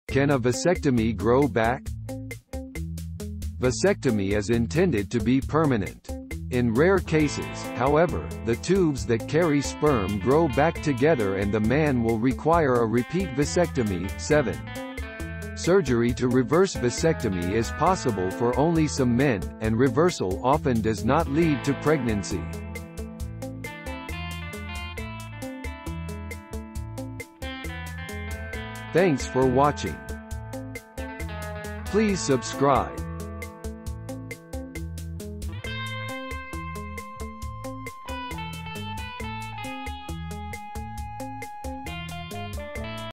Can a vasectomy grow back?
Vasectomy is intended to be permanent. In rare cases, however, the tubes that carry sperm grow back together and the man will require a repeat vasectomy. 7. ... Surgery to reverse vasectomy is possible for only some men, and reversal often does not lead to pregnancy.

Reference:
Questions and Answers About Vasectomy | Family Planning